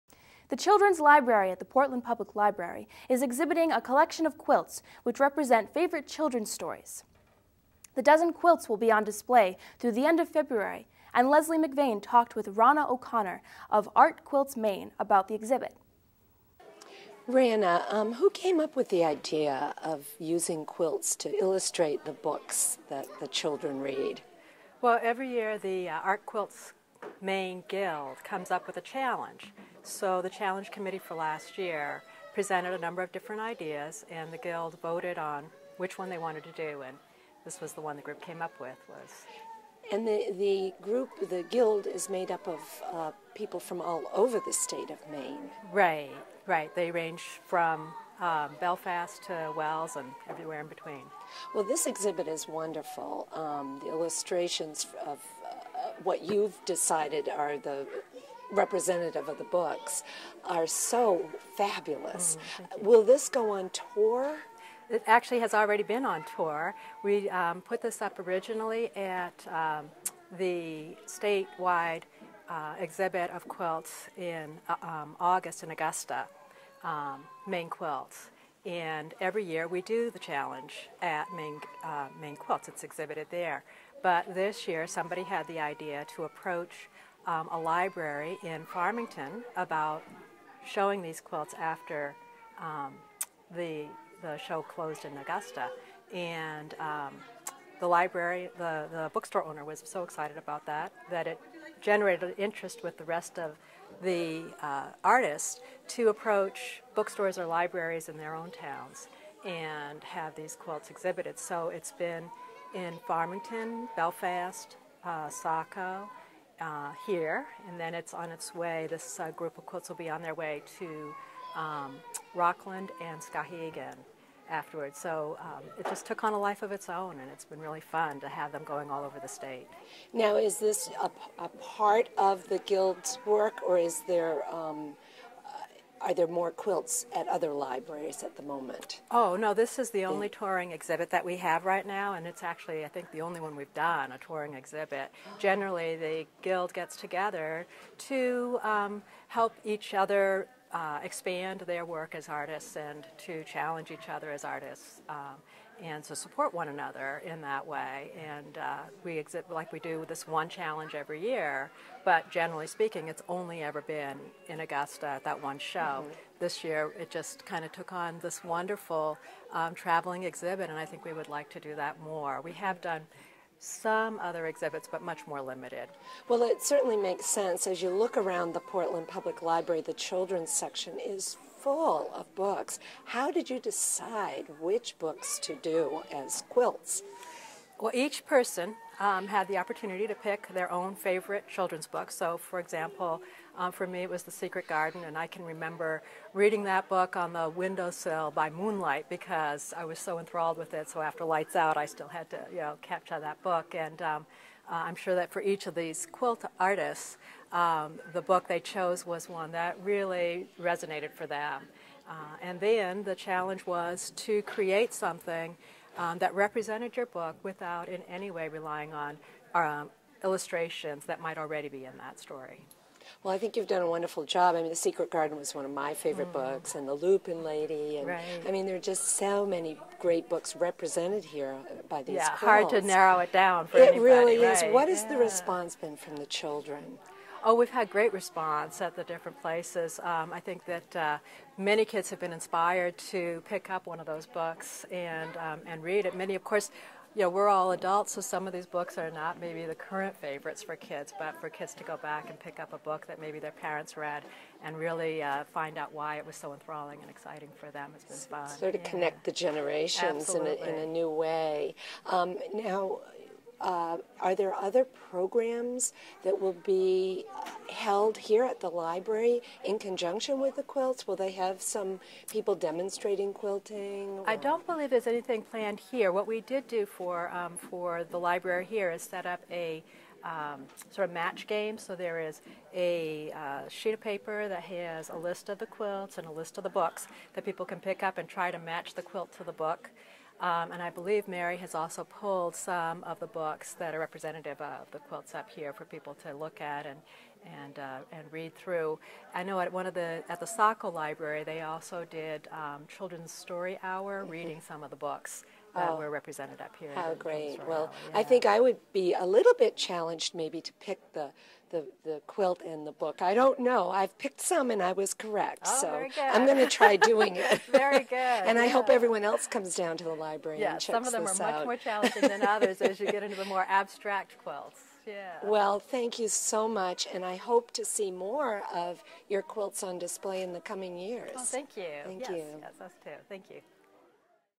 Portland Public Library exhibits "Art Quilts Maine".mov
"Art Quilts Maine" member, Rana O'Connor, talks about the exhibit at Portland Public Library.